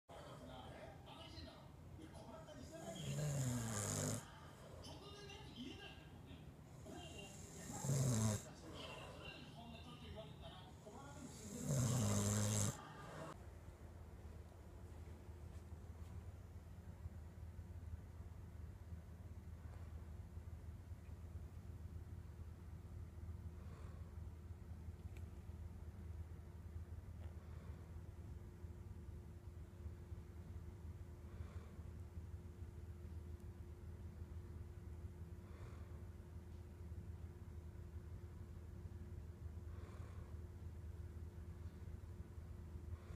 いびきにナステントは有効か？
私が仰向けで寝ているところを、妻に撮影してもらいました。あくまで睡眠の一時点での様子ですが、いびき改善に効果があるようです。

久留米市のみみ はな のどの専門家
はかたみち耳鼻咽喉科
24時間予約専用ダイヤル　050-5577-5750